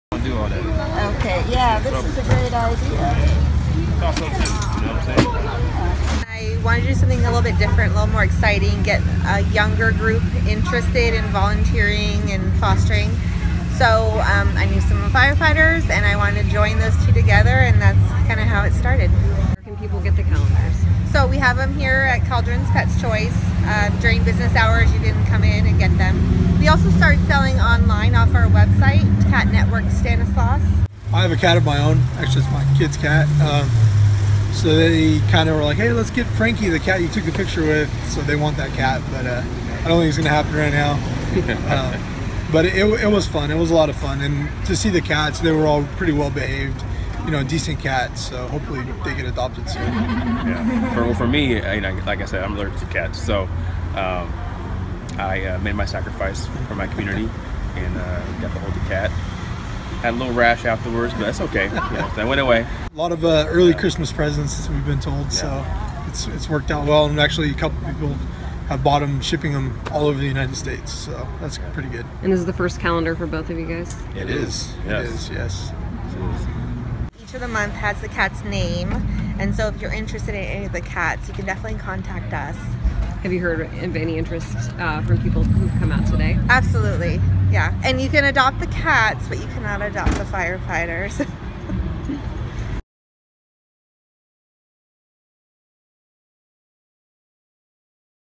Local firefighters pose for calendar to help cat rescue
Firefighters from around the region posed for a 2018 calendar to benefit the CatNetwork of Stanislaus. Some of the firefighters were  on hand Saturday to sign the calendars at Caldron's Pet's Choice in the Frontier Town shopping center on Tully Road, where the calendars are sold.